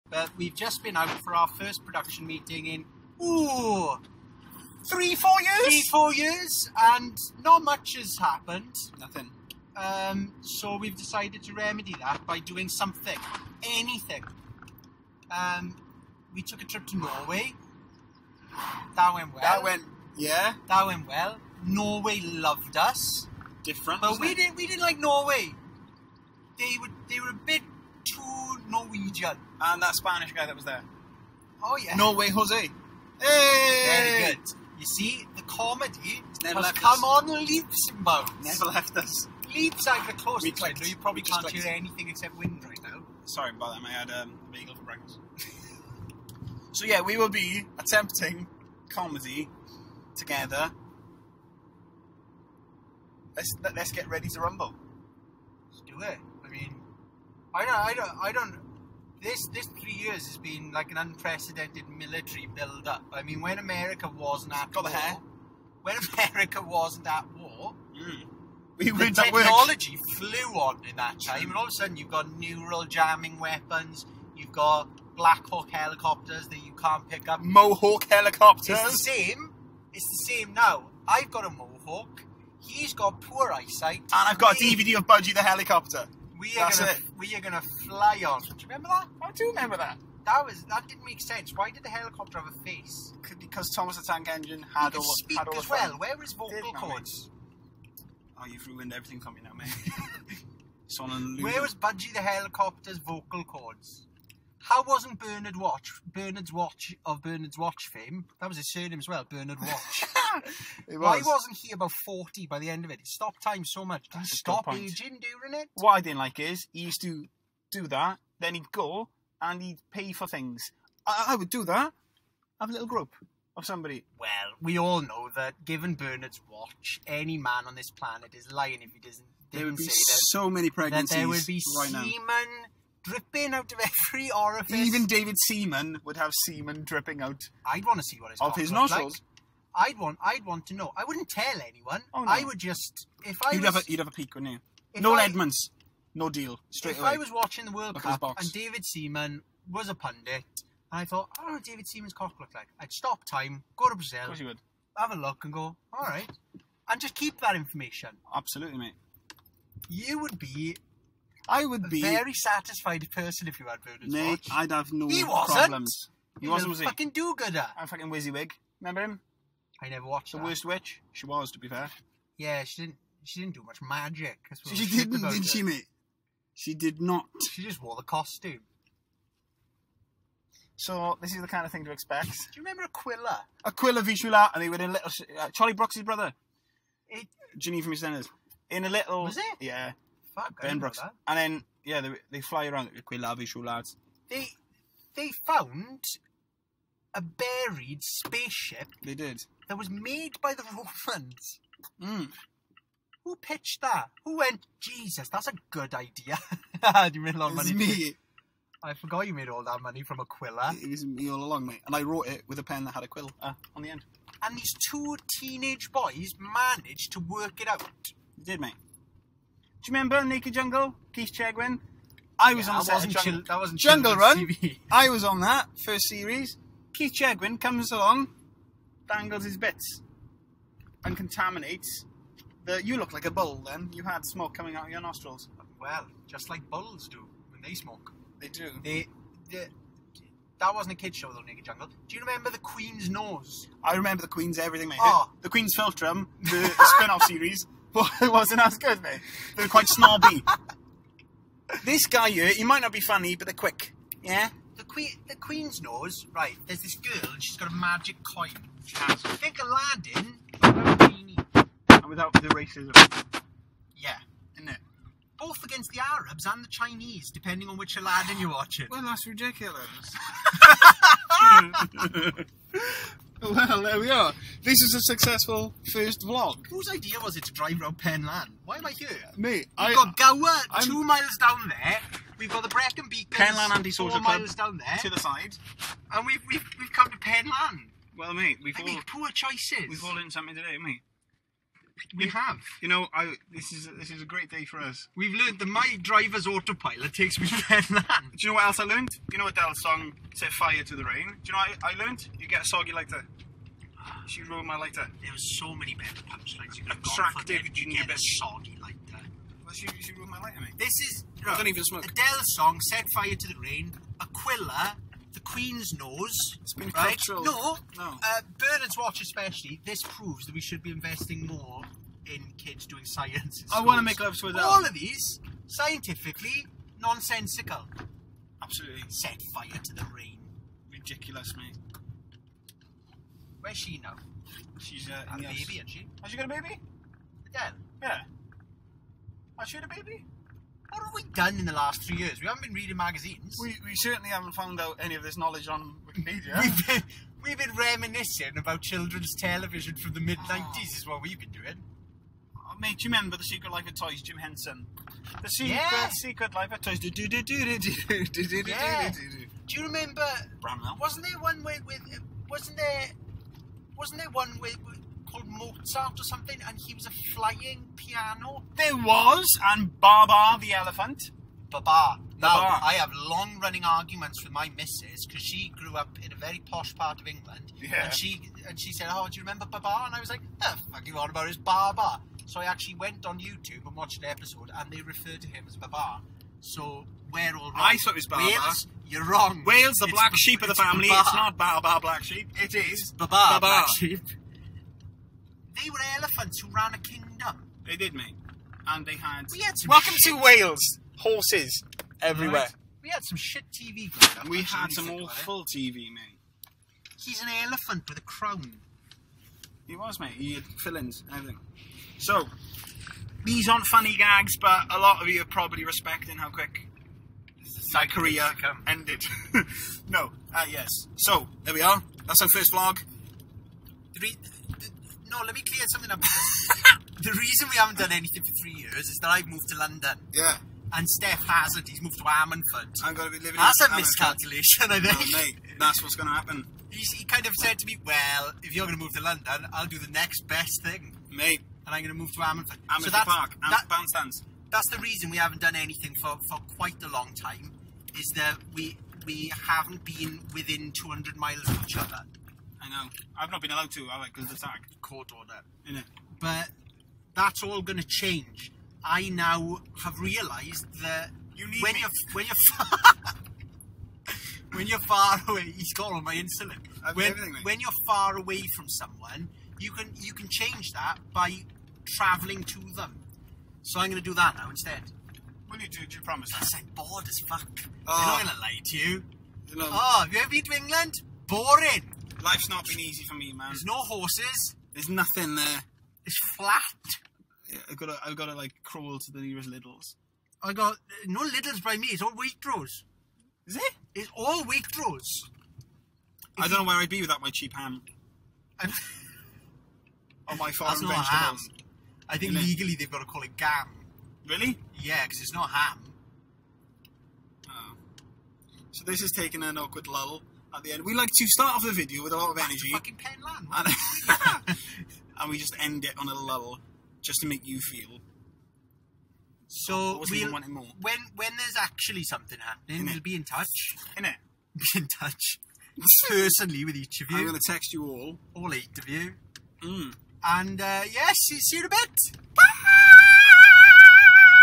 Stefan Pejic and Ceri Phillips (LE 122) reunite after 3 years
Welsh Comedy duo LE ONE TWO TWO (LE122)  meet up after 3 years of going their separate ways!